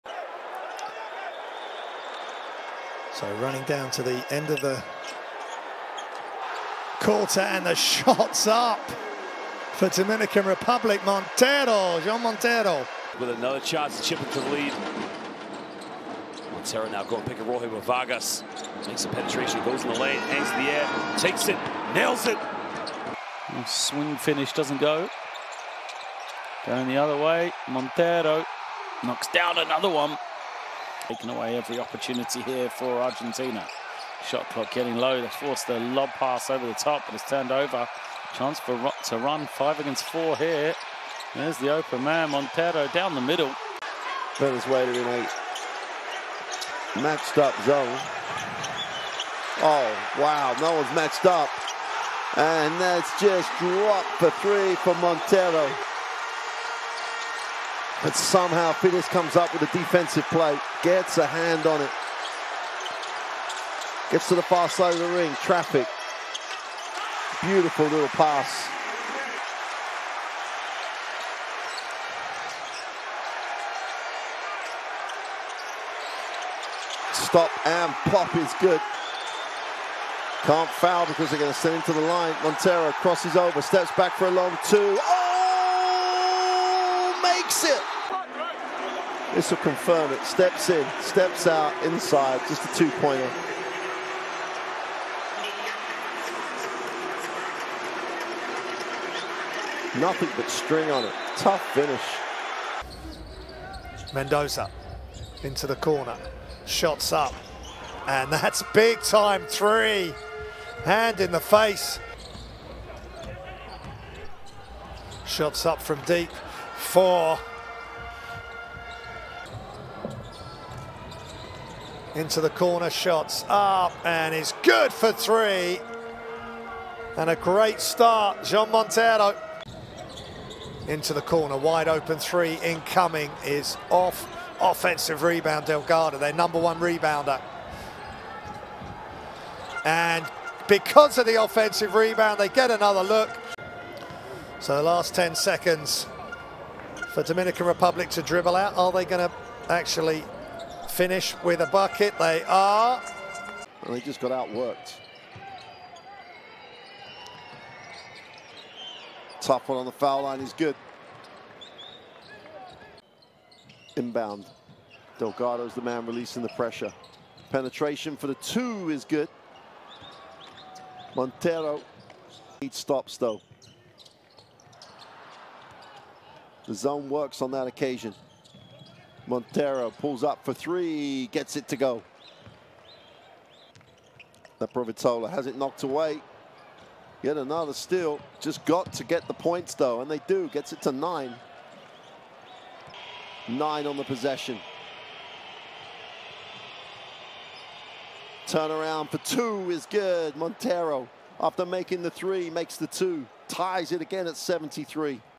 The 19-year-old who led his country to the World Cup: Jean Montero 🇩🇴
🇩🇴 Jean Montero guided Dominican Republic to the FIBA Basketball World Cup with his sensational performance throughout Americas Qualifiers. Watch his amazing highlights and get to know more about him 👀 🔥